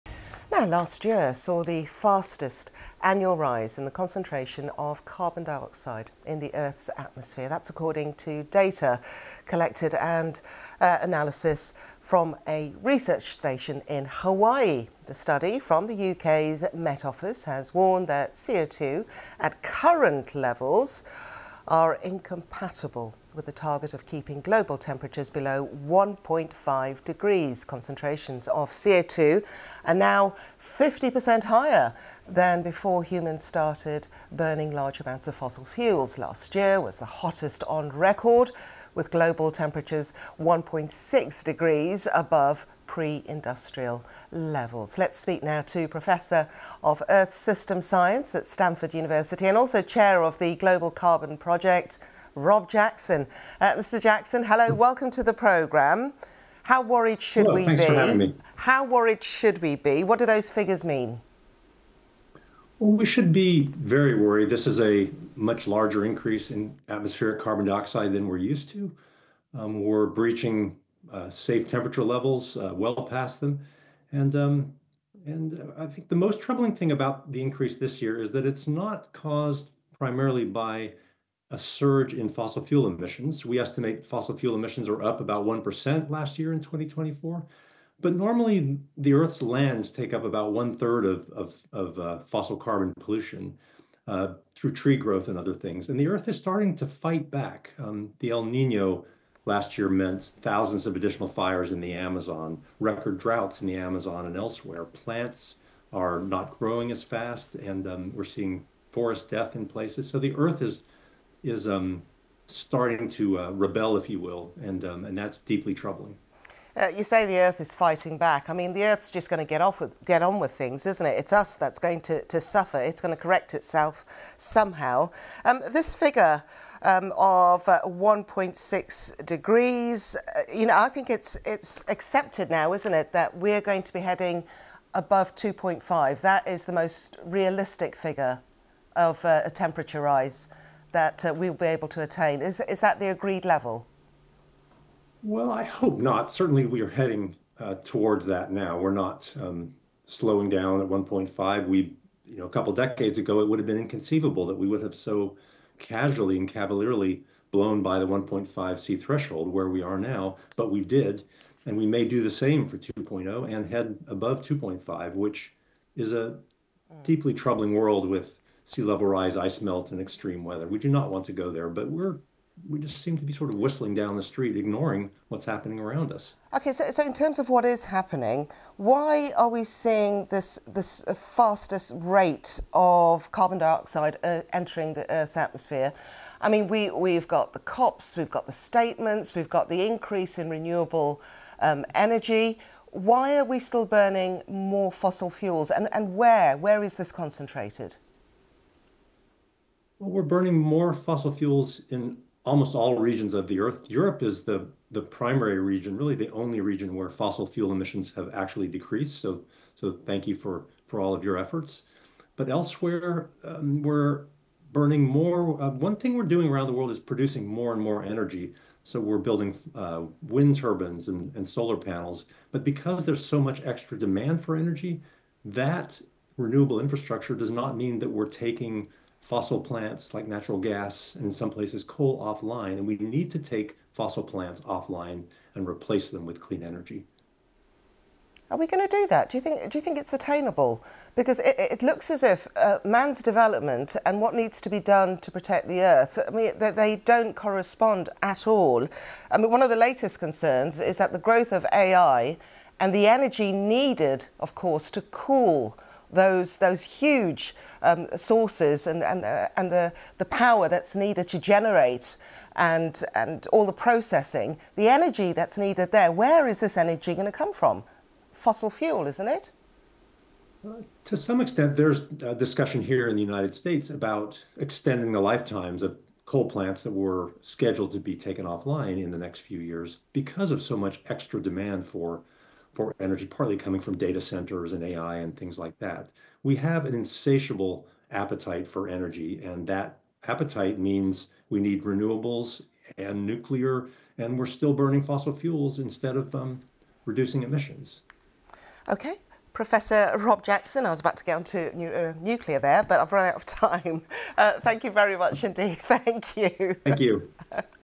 Climate change: Carbon dioxide rises more than ever in 2024 | BBC News
Levels of the most significant planet-warming gas in our atmosphere rose more quickly than ever previously recorded last year, scientists have said.

Concentrations of carbon dioxide (CO2) are now more than 50% higher than before humans started burning large amounts of fossil fuels.

Last year, fossil fuel emissions were at record highs, while the natural world struggled to absorb as much CO2 due to factors including wildfires and drought, so more accumulated in the atmosphere.

The rapid increase in CO2 is "incompatible" with the international pledge to try to limit global warming to 1.5C above pre-industrial levels, the Met Office said.